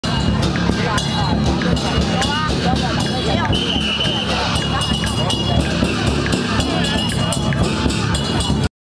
Xi'an - Song and Dance by South Gate (1)
Impromptu public singing and dancing by the south gate of Xian city wall.